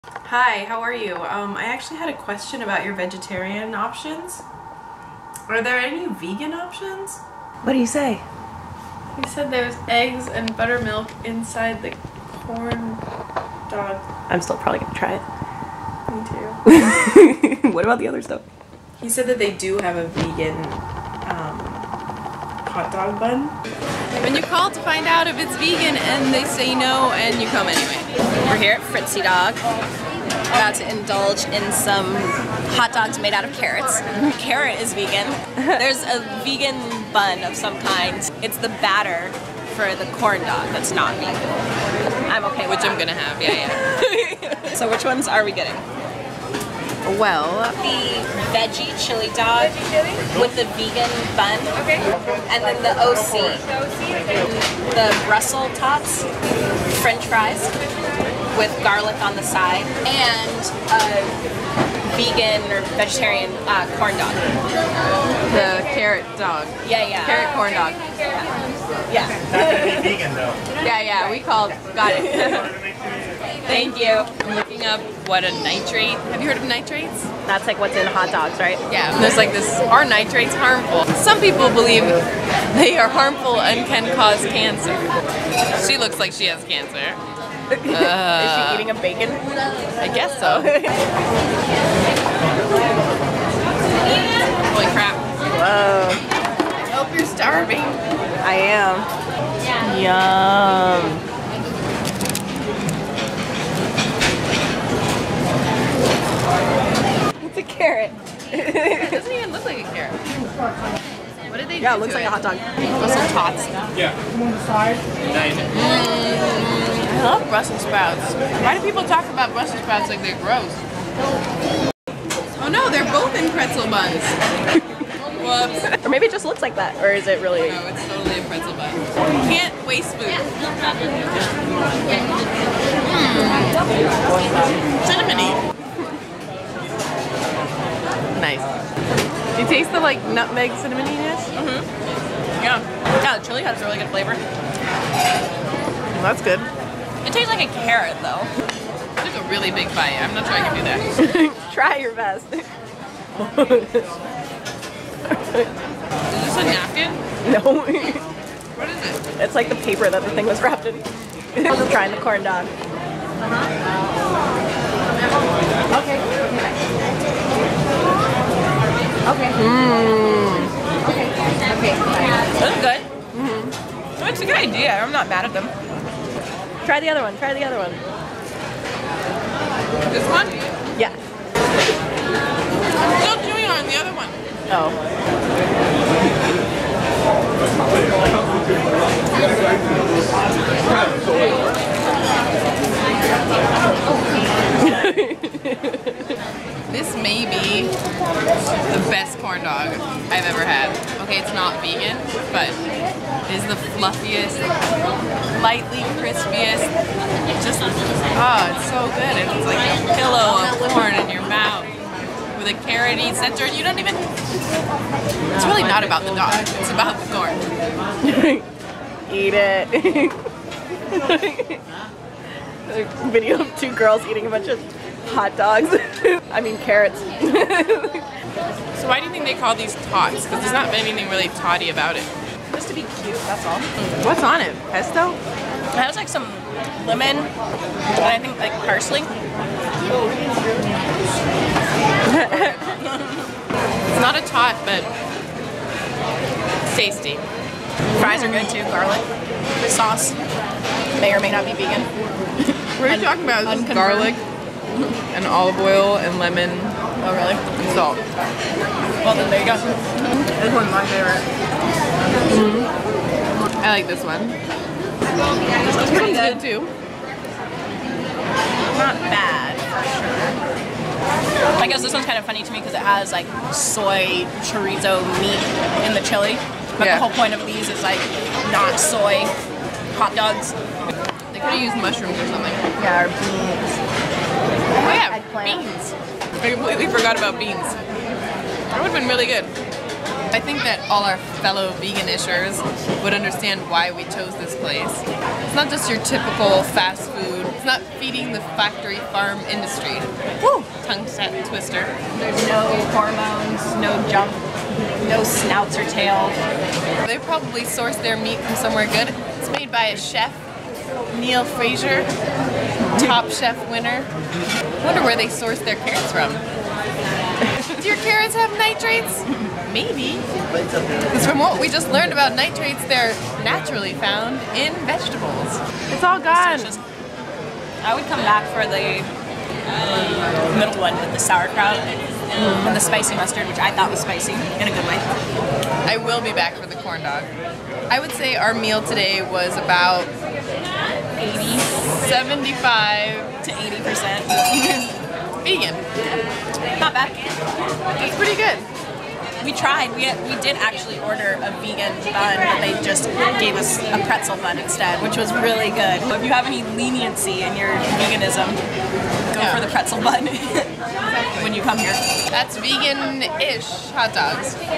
Vegan Hot Dogs / Veg Corn Dog. FRITZI DOG L.A. Review.
Our second Transitional Tuesday was at Fritzi Dog at the original farmers market in LA. These episodes focus on foods that are more about finding balance than health, (even though in this case carrots are healthier than hot dogs no question) these are the foods that help us make even more plant based choices without feeling like we are missing out.

PS- we recently found out the Pretzel Buns in this video were actually VEGAN so 2 out of 3 of our dogs were 100% Vegan. They are so delicious, go get you some!